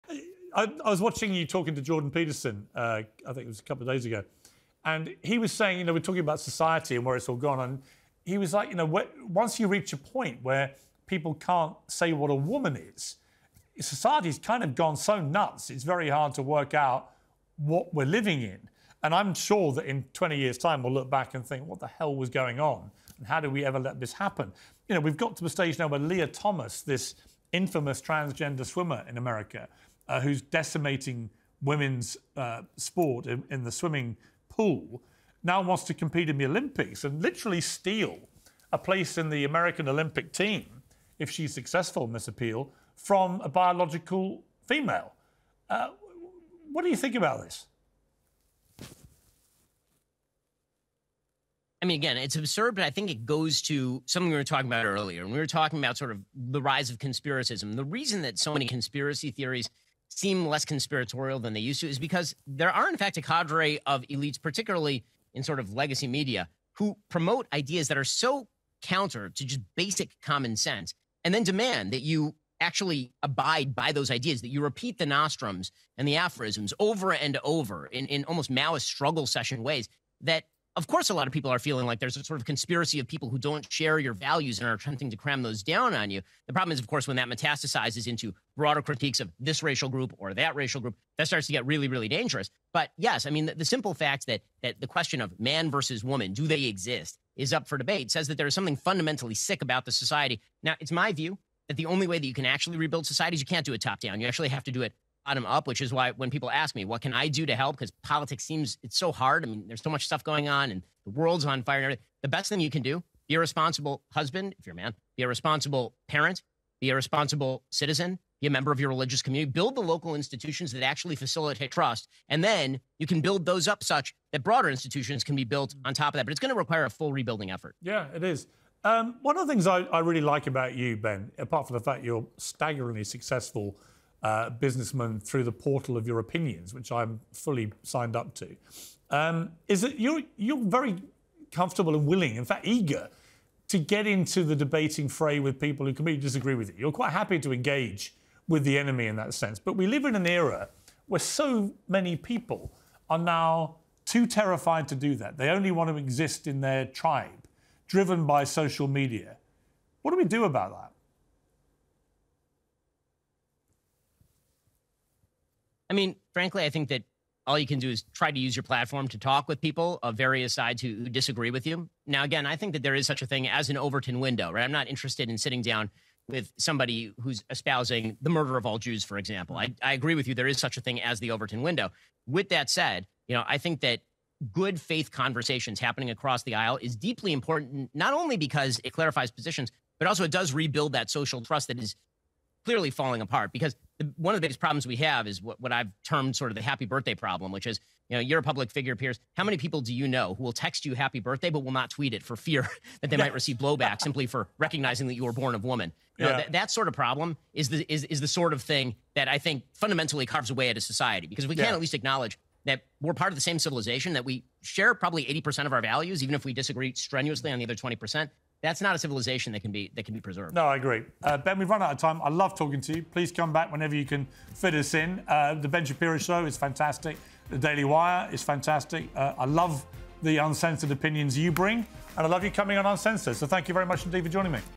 Ben Shapiro discusses why 'conspiracy theories' seem 'less conspiratorial'
Political commentator Ben Shapiro speaks about the apparent rise of “conspiracism” with Sky News Australia host Piers Morgan.

Mr Shapiro made the comments in a discussion about trans swimmer Lia Thomas’ bid to overturn a ban preventing her from competing in elite women's competitions, including the Olympics.

“The reason that so many conspiracy theories seem less conspiratorial than they used to is because there are in fact a cadre of elites, particularly in sort of legacy media, who promote ideas that are so counter to just basic common sense," he told Mr Morgan.

“And then demand that you actually abide by those ideas, that you repeat the nostrums and the aphorisms over and over in almost Maoist struggle session ways.

“That of course a lot of people are feeling like there’s a sort of conspiracy of people who don’t share your values and are trying to … cram those down on you."